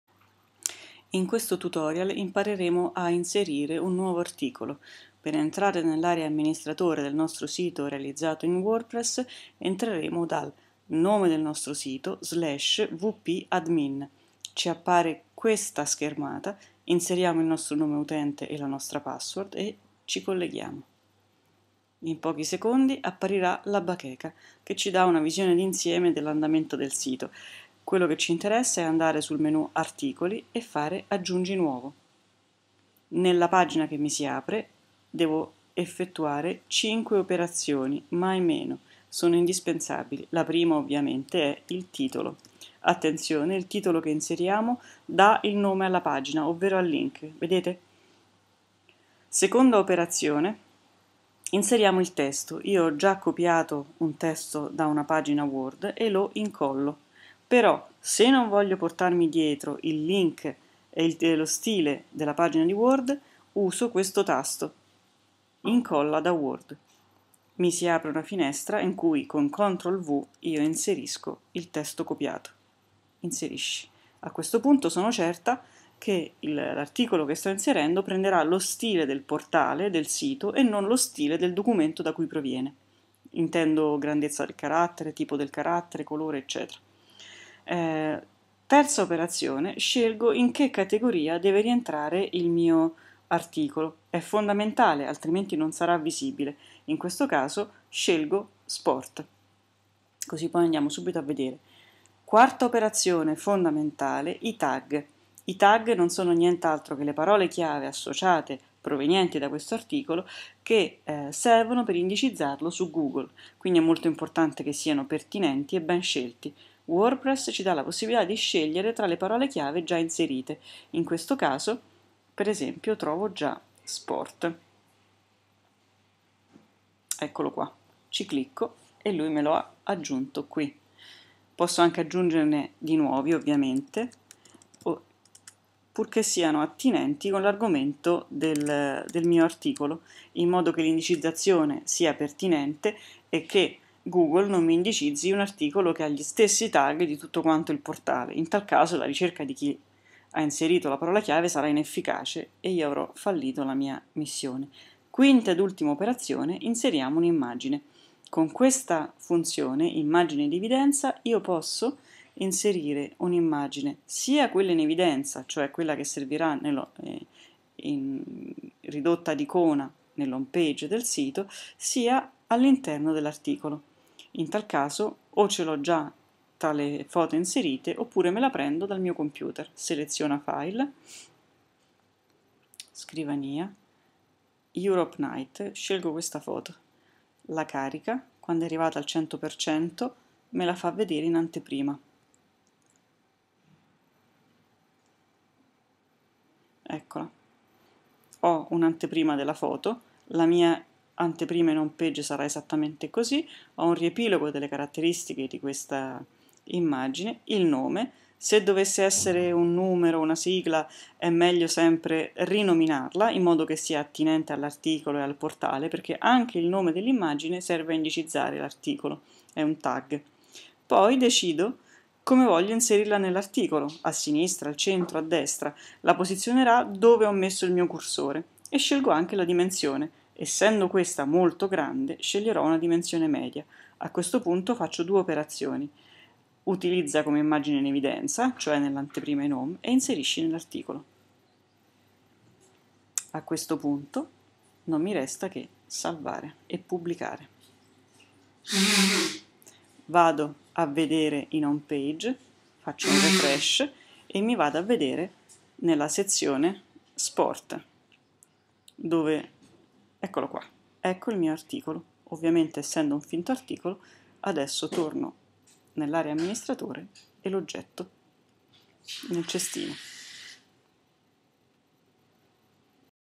Wordpress tutorial - Aggiungi nuovo articolo
Un breve tutorial per imparare a inserire un nuovo articolo (post) su un sito realizzato con wordpress